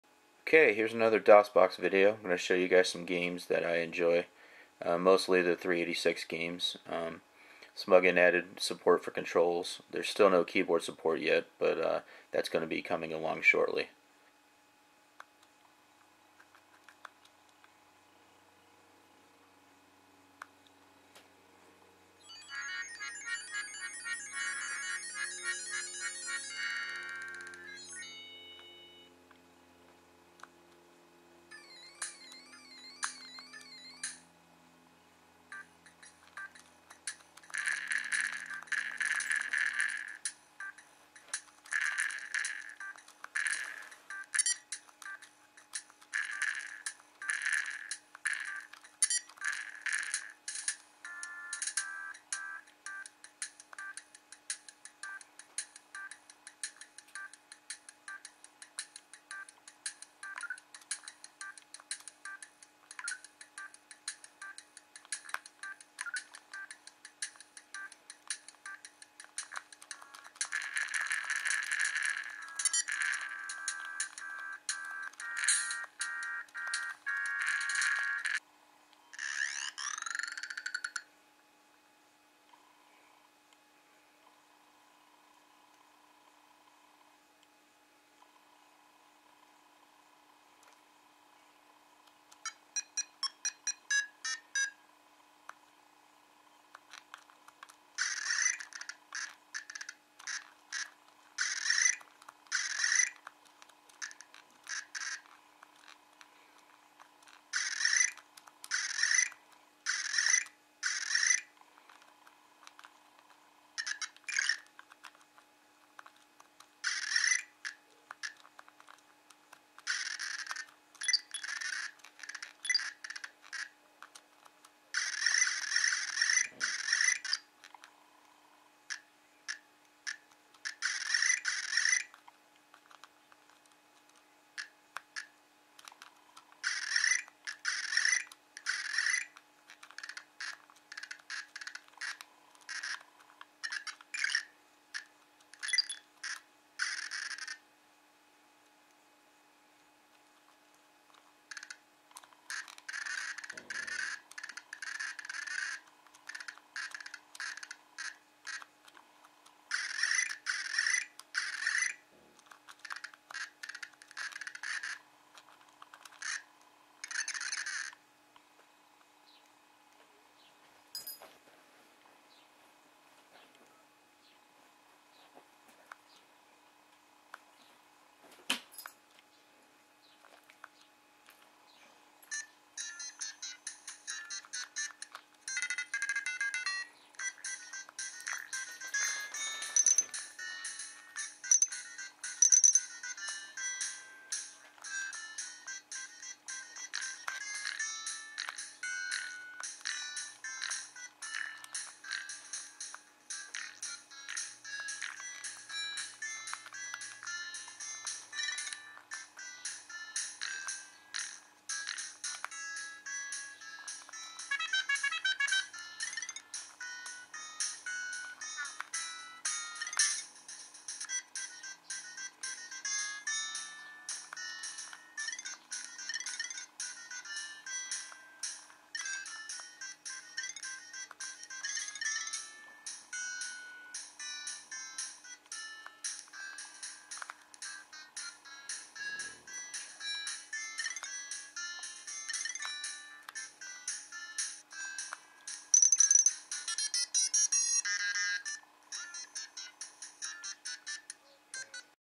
More DOSbox on the GCW
Here are few ega games for you guys. These are all 386 games but some of my favs. Don't wory some bigger games will be shown shortly.